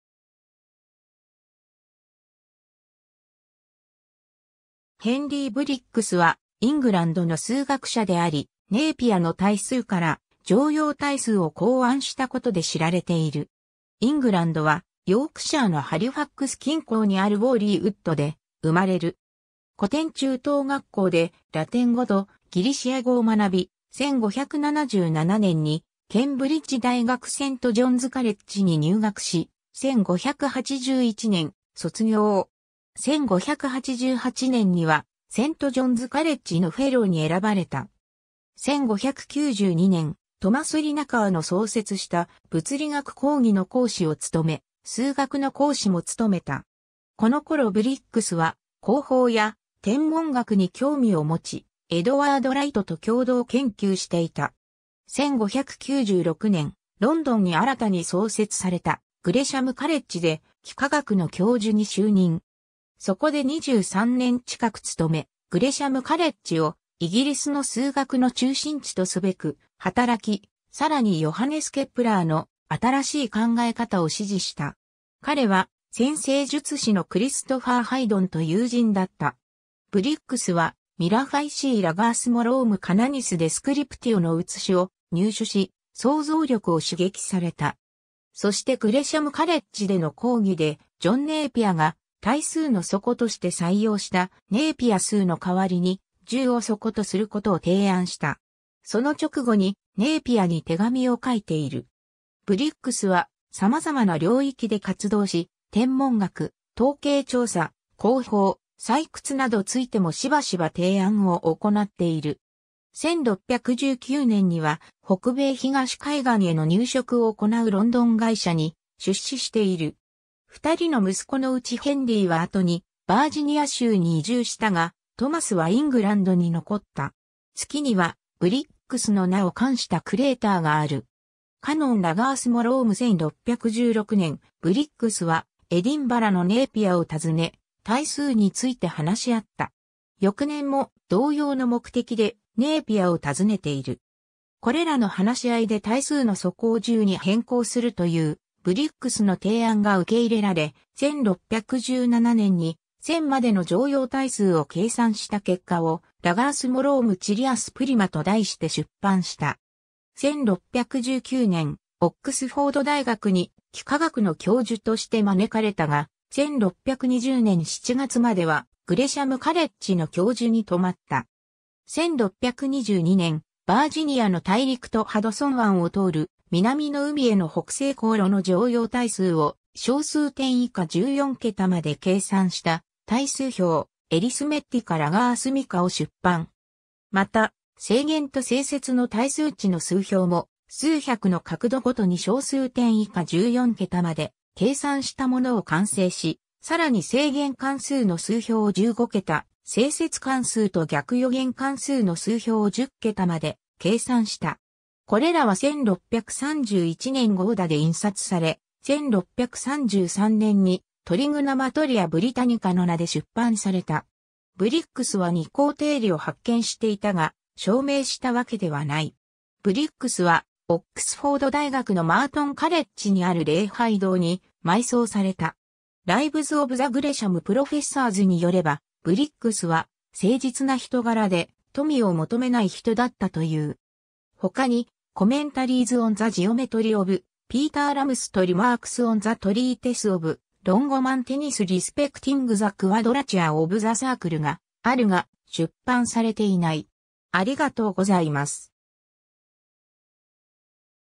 ヘンリー・ブリッグス
イングランド・ハリファクス出身の人物
ヘンリー・ブリッグス（英: Henry Briggs, 1561年2月 - 1630年1月26日）は、イングランドの数学者であり、ネイピアの対数から常用対数を考案したことで知られている。
 イングランドはヨークシャーのハリファックス近郊にある Warley Wood で生まれる。
古典中等学校でラテン語とギリシア語を学び、1577年にケンブリッジ大学セント・ジョンズ・カレッジに入学し、1581年卒業。
1588年にはセント・ジョンズ・カレッジのフェローに選ばれた。
1592年、トマス・リナカーの創設した物理学講義の講師を務め、数学の講師も務めた。
このころブリッグスは航法や天文学に興味を持ち、エドワード・ライトと共同研究していた。
1596年、ロンドンに新たに創設されたグレシャム・カレッジで幾何学の教授に就任。
そこで23年近く務め、グレシャム・カレッジをイギリスの数学の中心地とすべく働き、さらにヨハネス・ケプラーの新しい考え方を支持した。
彼は占星術師のクリストファー・ハイドンと友人だった。
ブリッグスは Mirifici Logarithmorum Canonis Descriptio（ジョン・ネイピアの論文）の写しを入手し、想像力を刺激された。
そしてグレシャム・カレッジでの講義で、ジョン・ネイピアが対数の底として採用したネイピア数の代わりに、10を底とすることを提案した。
その直後にネイピアに手紙を書いている。
ブリッグスは様々な領域で活動し、天文学、統計調査、航法、採掘などついてもしばしば提案を行っている。
1619年には、北米東海岸への入植を行うロンドン会社（London Company）に出資している。
2人の息子のうちヘンリーは後にバージニア州に移住したが、トマスはイングランドに残った。
月にはブリッグスの名を冠したクレーターがある。
 Canon logarithmorum 1616年、ブリッグスはエディンバラのネイピアを訪ね、対数について話し合った。
翌年も同様の目的でネイピアを訪ねている。
これらの話し合いで対数の底を10に変更するというブリッグスの提案が受け入れられ、1617年に1000までの常用対数を計算した結果を Logarithmorum Chilias Prima と題して出版した。
1619年、オックスフォード大学に幾何学の教授として招かれたが、1620年7月まではグレシャム・カレッジの教授に留まった。
 1622年、『バージニアの大陸とハドソン湾を通る、南の海への北西航路』 （Northwest Passage to the South Seas, through the Continent of Virginia and Hudson's Bay という小冊子を出版。1624年には、3万個の自然数（1から20,000までと90,000から100,000まで）の常用対数を小数点以下14桁まで計算した対数表 Arithmetica Logarithmica を出版。
また、正弦と正接の対数値の数表も数百の角度ごとに小数点以下14桁まで計算したものを完成し、さらに正弦関数の数表を15桁、正接関数と逆余弦関数の数表を10桁まで計算した。
これらは1631年ゴーダで印刷され、1633年に Trigonometria Britannica の名で出版された。
ブリッグスは二項定理を発見していたが、証明したわけではない。
 ブリッグスはオックスフォード大学のマートン・カレッジにある礼拝堂に埋葬された。
Lives of the Gresham Professors によれば、ブリッグスは誠実な人柄で、富を求めない人だったという。
 他に、Commentaries on the Geometry of Peter Ramus と Remarks on the Treatise of Longomontanus respecting the Quadrature of the Circle があるが、出版されていない。